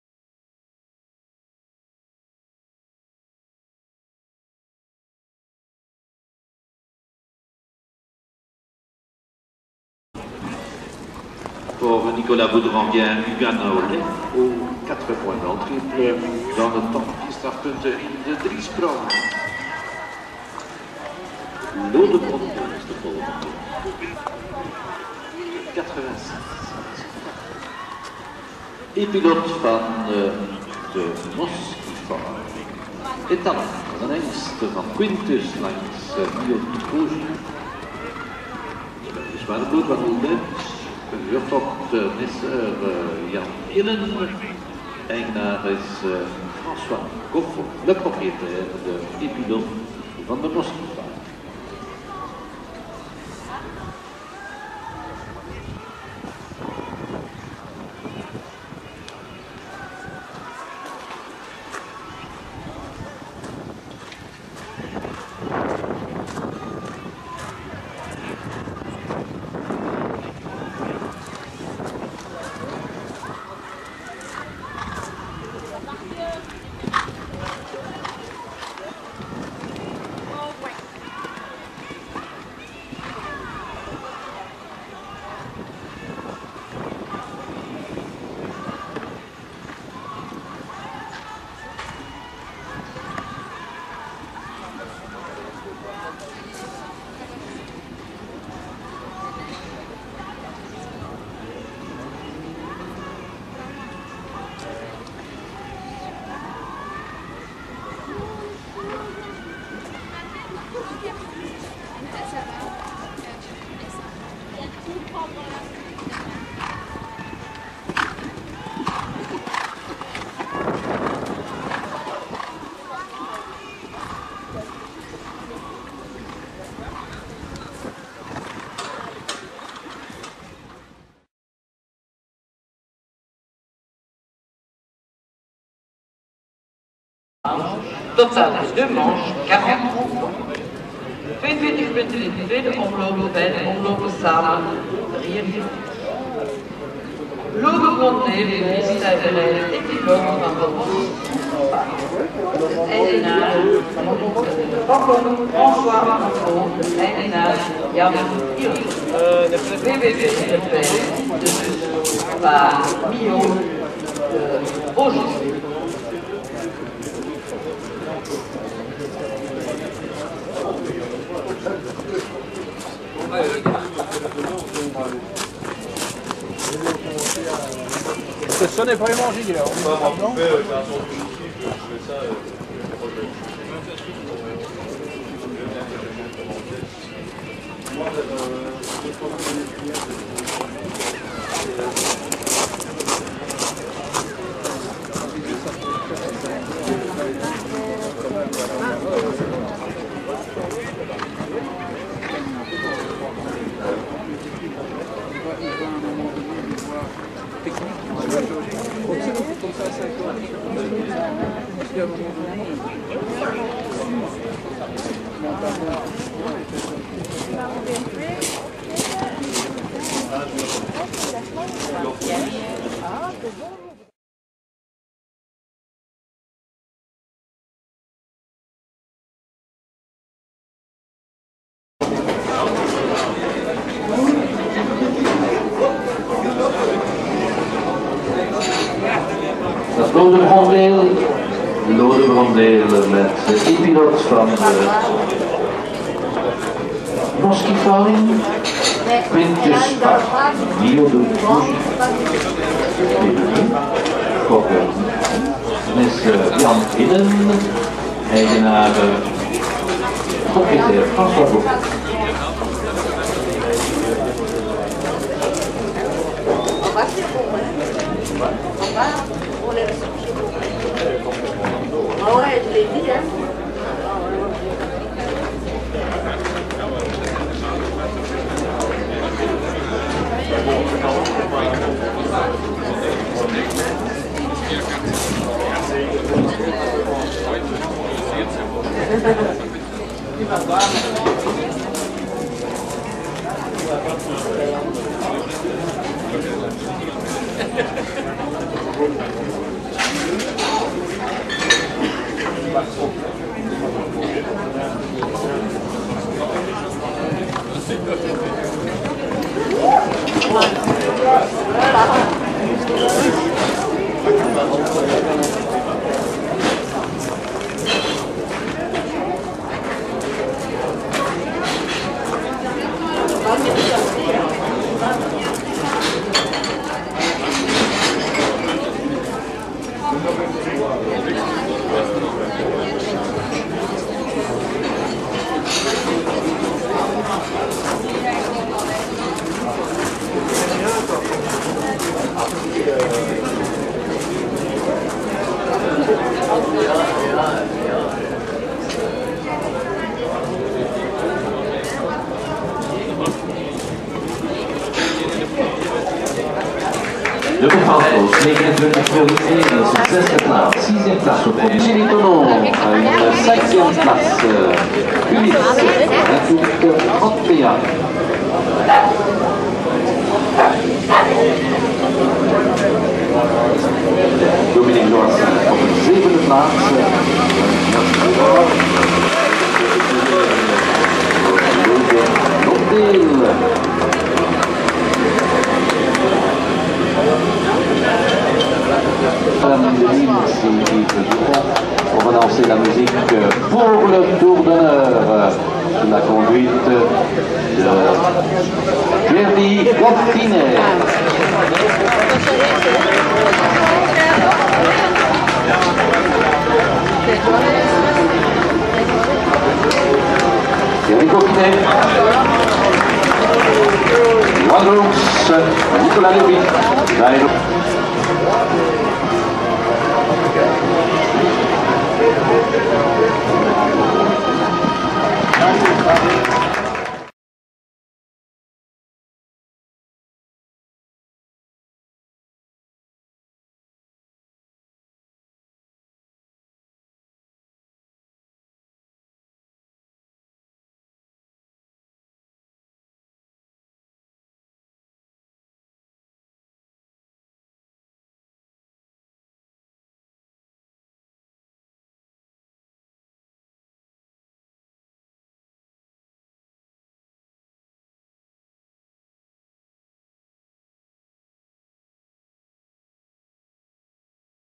2009-08-12 Epilot Belgisch Kampioenschap Young Horses in Gesves-Belgium
Stallion EPILOT v/d Moskifarm zorgt voor de verrassing in Gesves:
Op dit Belgisch kampioenschap voor jonge paarden liep hij 3 omlopen zonder ook maar één springfout te maken!!!
Alleen één puntje wegens tijdsoverschrijding in de 1st. omloop hielden hem uit de grote finale wat terecht spijtig was voor deze hengst.
Als besluit hield hij er toch een 7de plaats aan over in de Consolation Omloop en dit op 168 deelnemers.
Zijn vader Quintus zou het niet beter gedaan hebben op 5 jaar!!